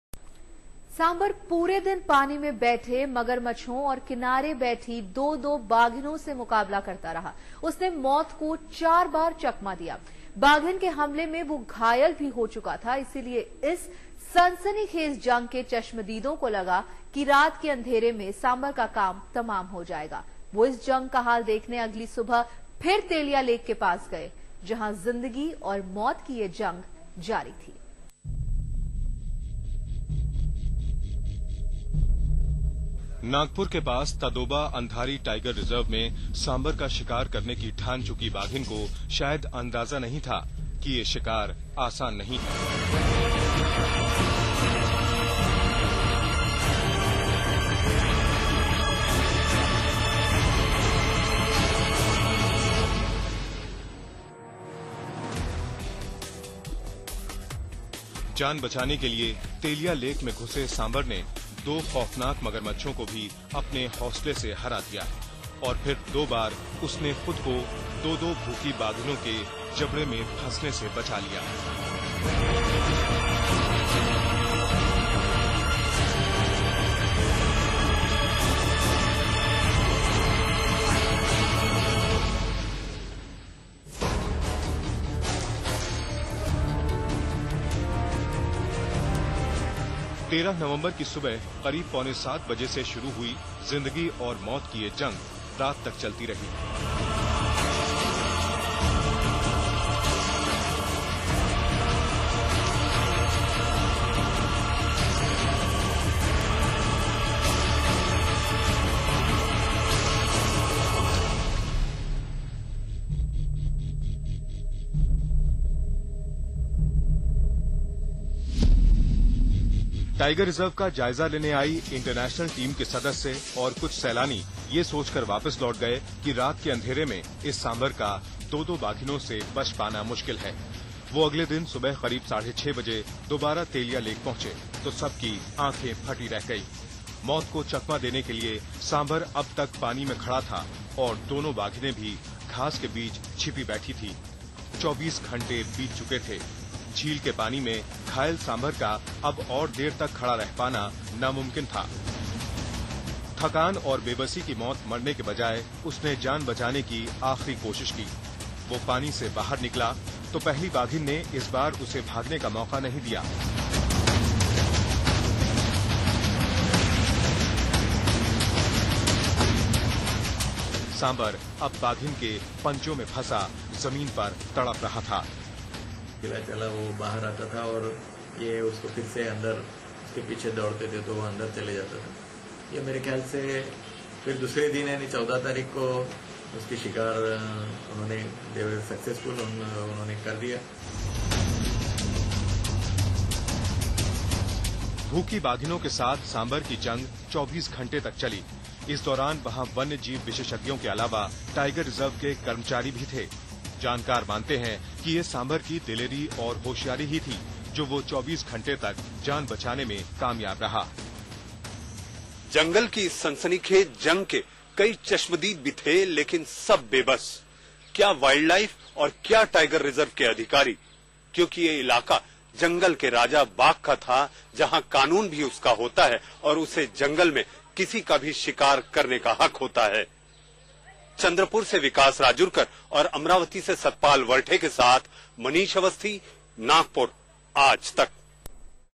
Sambhar deer surrenders to tigresses. Part 1 of 5
Watch how a sambhar deer surrenders to tigresses in Tadoba Tiger Reserve in Nagpur.